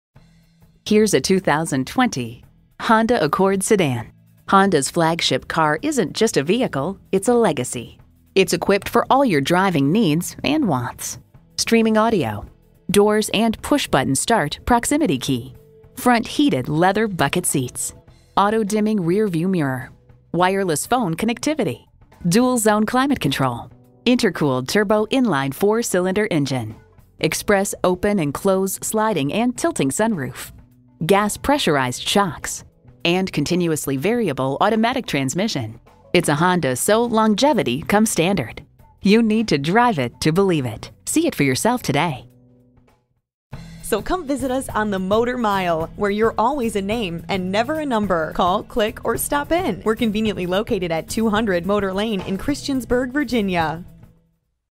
Used 2020 Honda Accord Christiansburg VA Blacksburg, VA X66145 - SOLD
Year: 2020
Make: Honda
Model: Accord
Trim: 4dr Car
Engine: 1.5L 4 Cylinder Engine
Transmission: CVT Transmission
Color: Champagne Frost Pearl
Interior: Ivory
Mileage: 57585
Stock #: X66145
VIN: 1HGCV1F54LA102098